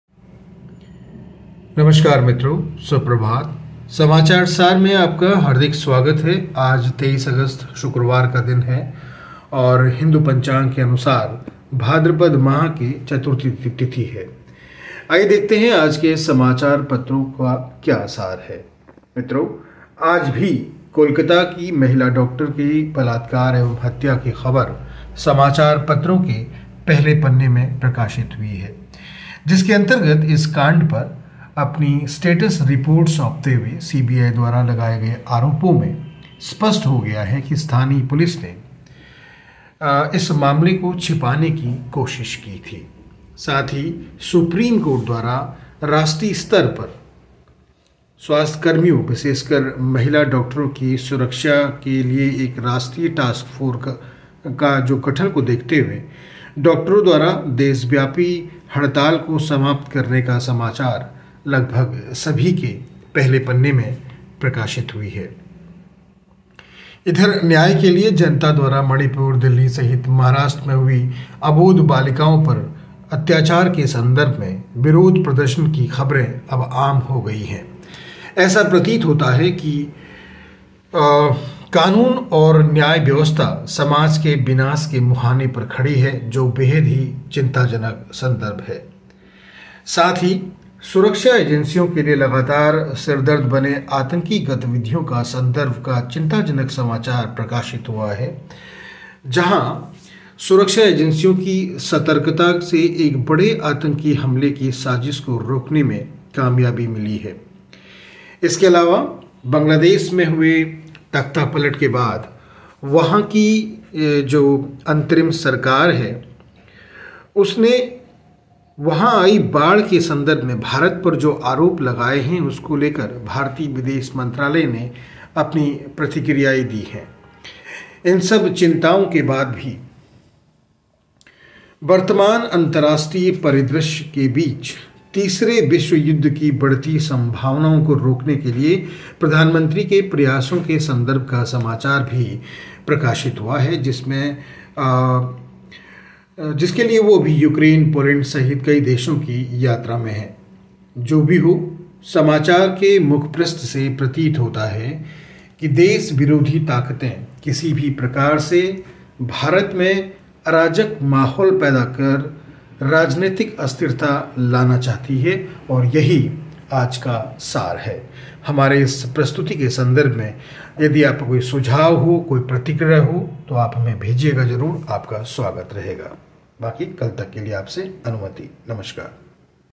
न्याय के लिए प्रदर्शन: कानून व्यवस्था का चीरहरण
आज भी कोलकाता के महिला डॉक्टर की बलात्कार एवं हत्या की खबर समाचार पत्रों के पहले पन्ने में प्रकाशित हुई है जिसके अंतर्गत इस कांड पर अपनी स्टेटस रिपोर्ट सौंपते हुए सीबीआई द्वारा लगाए गए आरोप में स्पष्ट हो गया है की स्थानीय पुलिस ने इस  मामले को छिपाने की कोशिश की थी. साथ ही सुप्रीम कोर्ट द्वारा राष्ट्रीय स्तर पर स्वास्थ्य कर्मियों, विशेषकर महिला डॉक्टरों की सुरक्षा के लिए एक राष्ट्रीय टास्क फोर्स के गठन को देखते हुए, डॉक्टरों द्वारा देशव्यापी हड़ताल को समाप्त करने का सामचार लगभग सभी के पहले पन्ने पर प्रकाशित हुई है